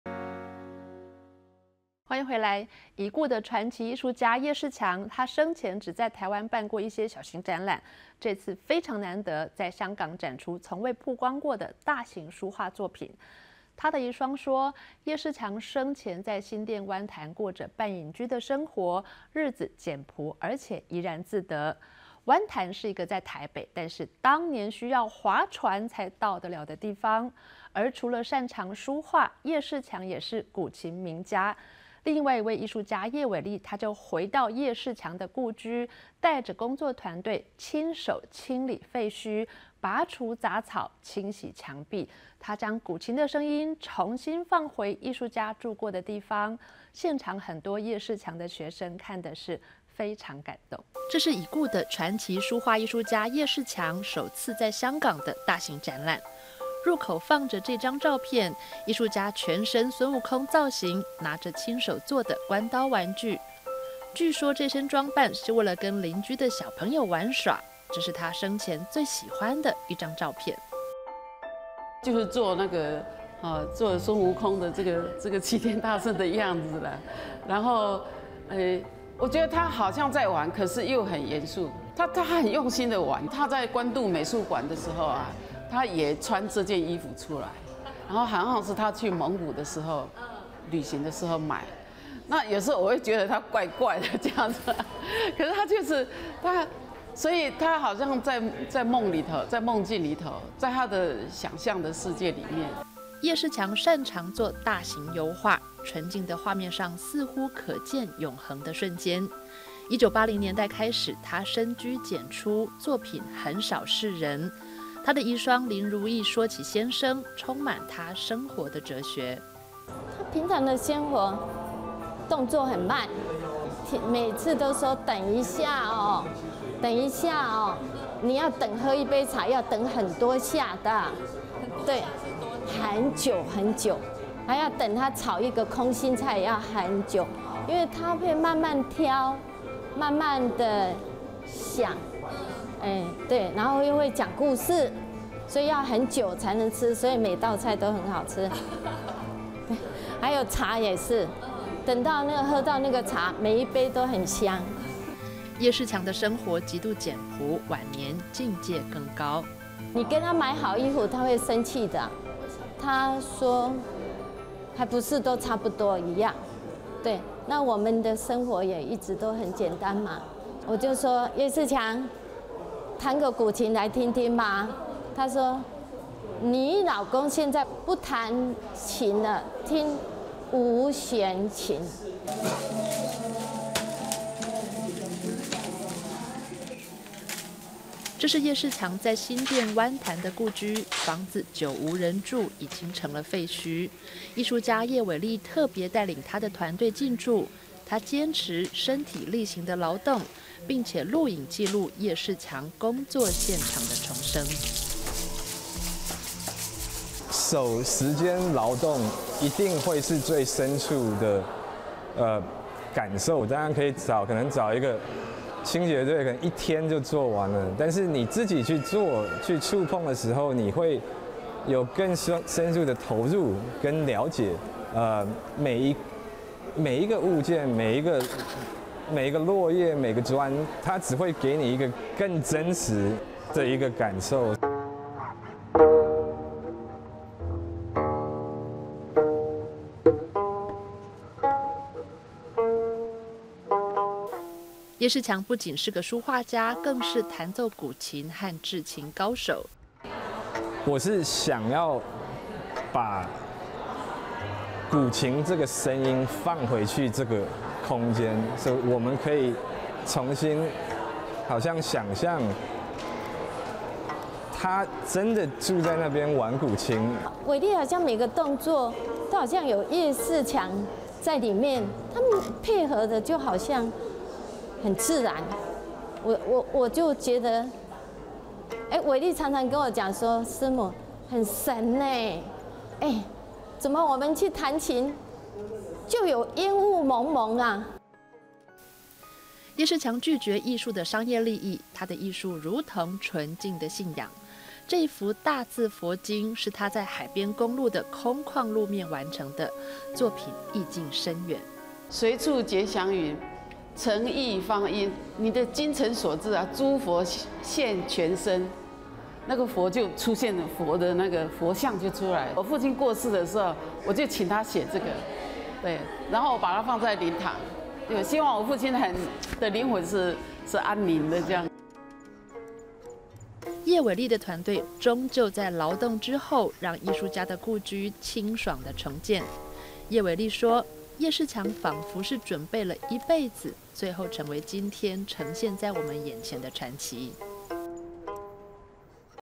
《藝想世界》已故傳奇藝術家葉世強 香港首展
已故傳奇藝術家葉世強，生前只在台灣辦過一些小型展覽，
這次非常難得，在香港展出從未曝光過的大型書畫作品。
他的遺孀說，葉世強生前在新店灣潭過著半隱居的生活，
日子簡樸且怡然自得。
灣潭是一個在台北，但是當年需要划船才到得了的地方。
除了擅長書畫，葉世強也是古琴名家。另外一位藝術家葉偉立，
回到葉世強隱居的新店灣潭，帶著工作團隊親手清理廢墟，
拔除雜草、清洗牆壁。
將古琴的聲音，重新放回藝術家住過的地方。現場很多葉世強的學生，看得非常感動。